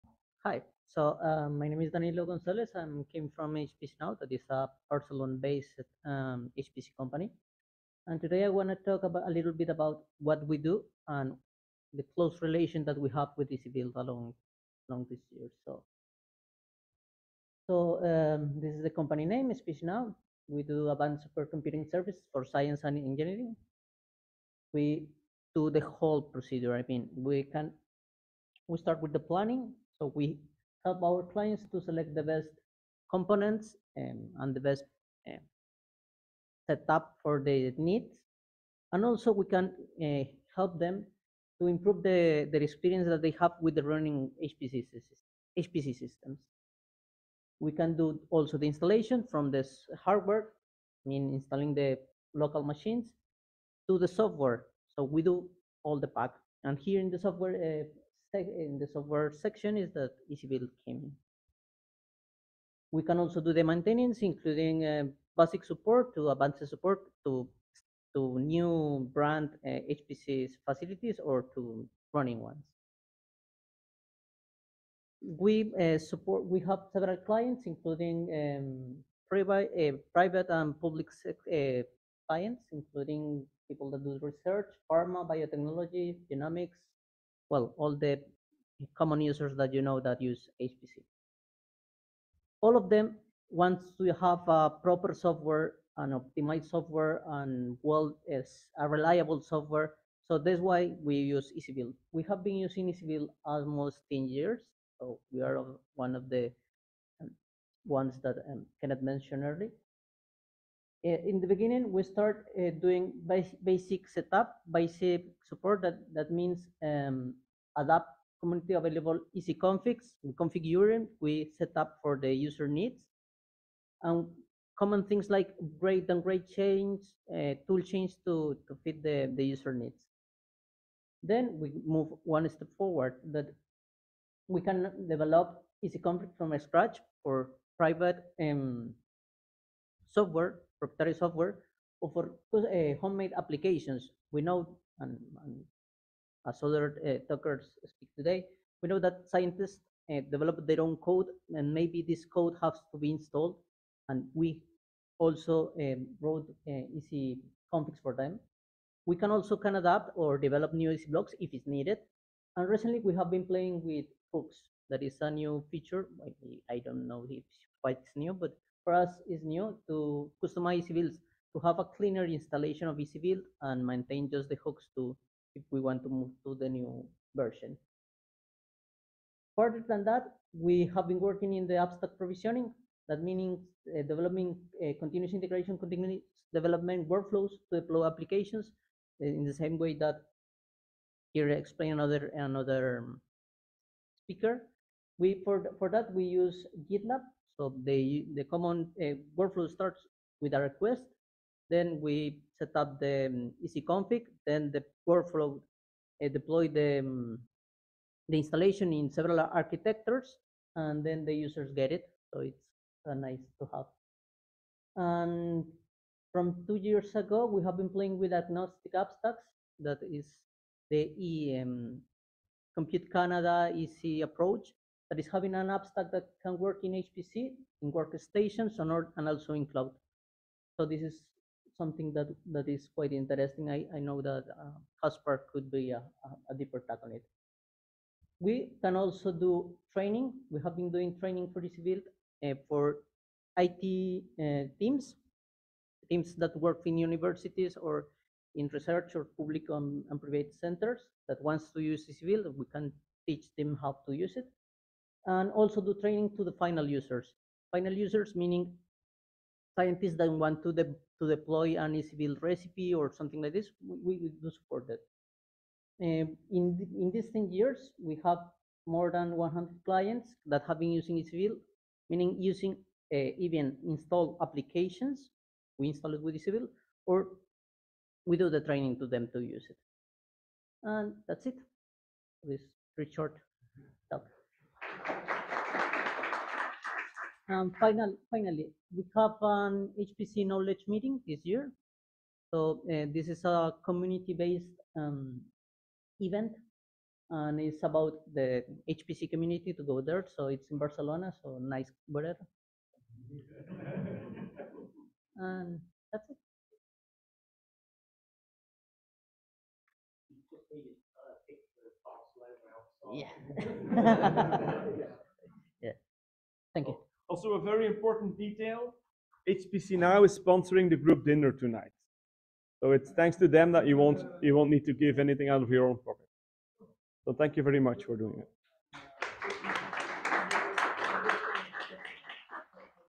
HPCNow! and EasyBuildby Danilo Gonzalez (HPCNow!)
by Danilo Gonzalez (HPCNow!) at the 8th EasyBuild User Meeting (EUM'23,

Abstract:

HPCNow! can help you with EasyBuild!